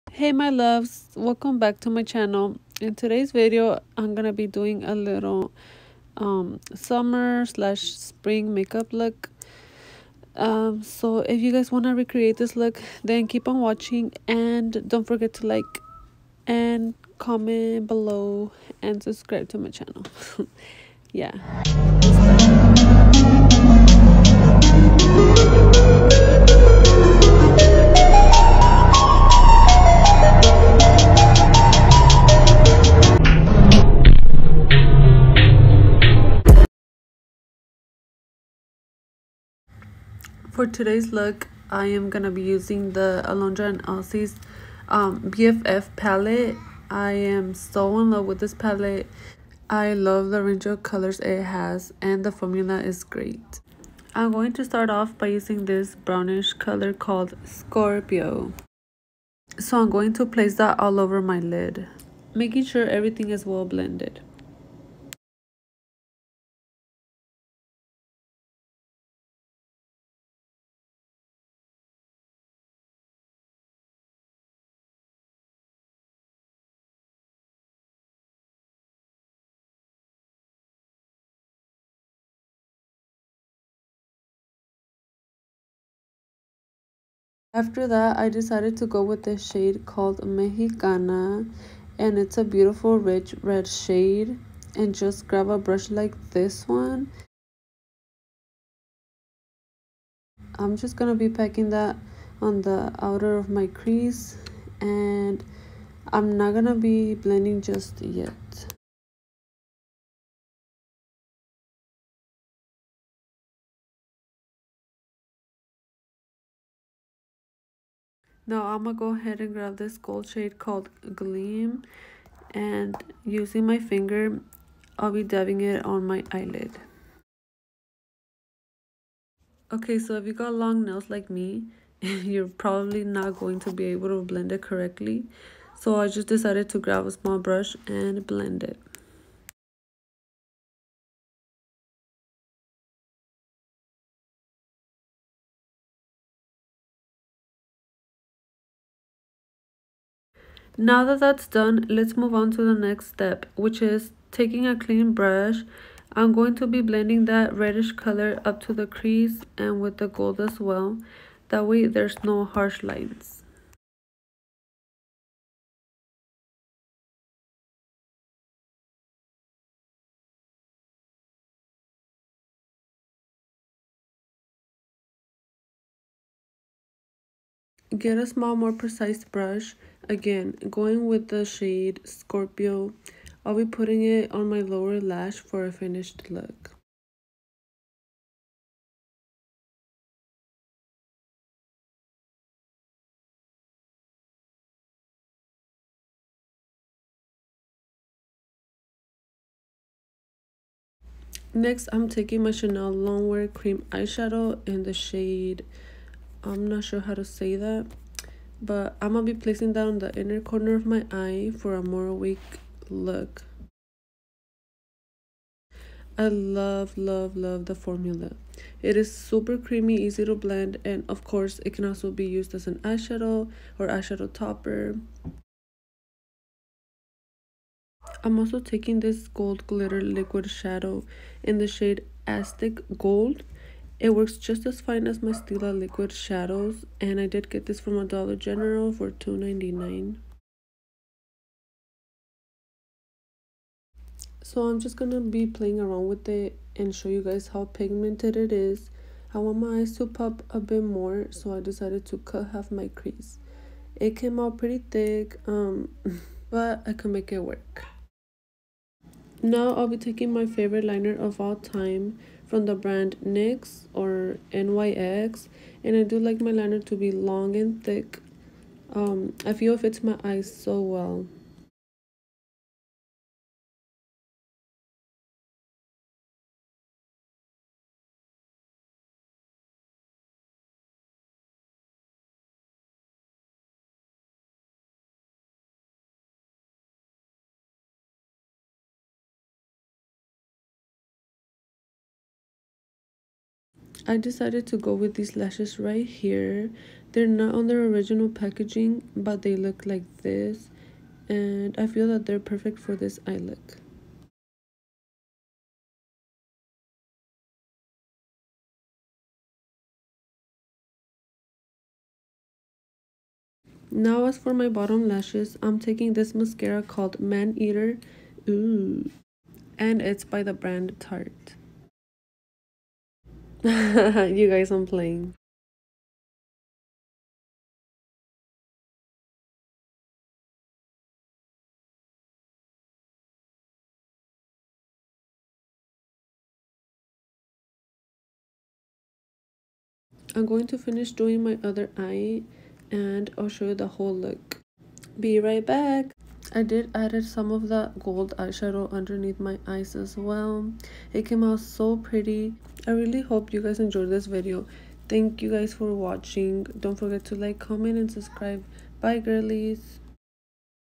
Quick Makeup Tutorial using *Alondra & Elsy’s BFF* pallet!
In today’s video I’ll be doing a quick makeup tutorial using Alondras & Elsy’s new BFF pallet from BH cosmetics.. I love the shades it has & I love how inexpensive this pallet is! Talk about Boujie on a budget 😜